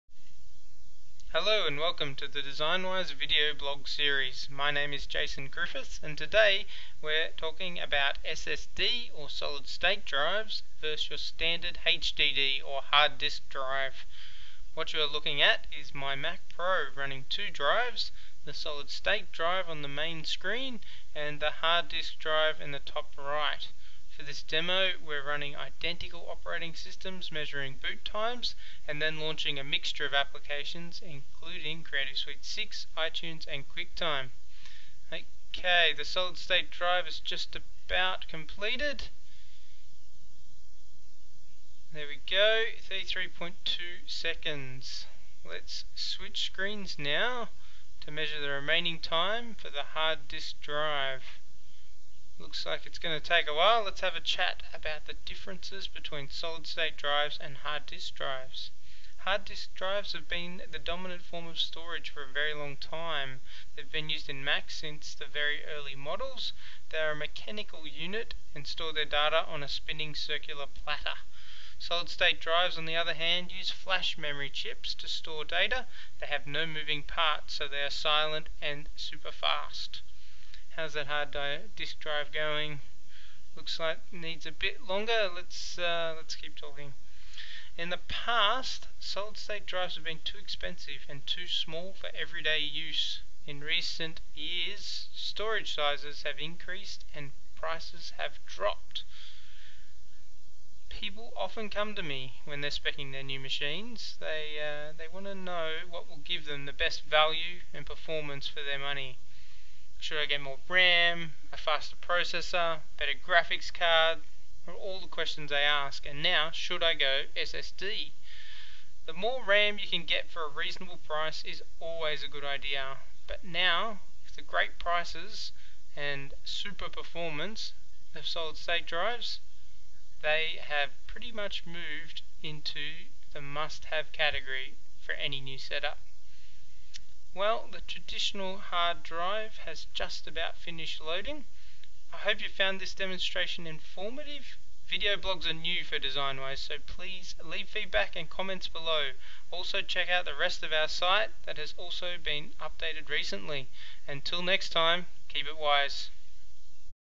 Solid State Drive (SSD) versus Hard Disc Drive - Speed Test
Mac System boot and software stat up on two identical systems with one big difference.
System 1 is operating off a Solid State Drive and System 2 is operating off a traditional Hard Drive.